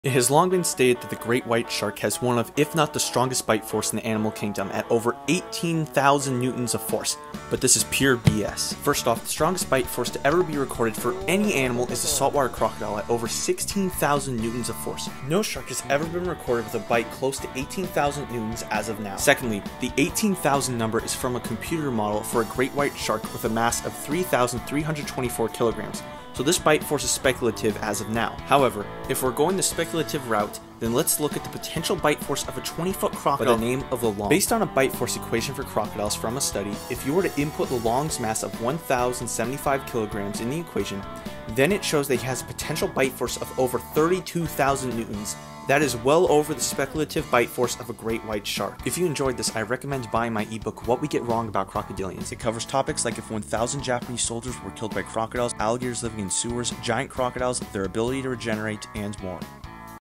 Shark vs. Crocodile: Who Has a Stronger Bite
A match up that is commonly talked about is a fight between a shark and a crocodile, or more specifically the Great White Shark vs. the Saltwater Crocodile. Both are large, aquatic predators, but who has the stronger bite? Watch the video to find out.

Huge Sharks, Monster Sharks, Large Sharks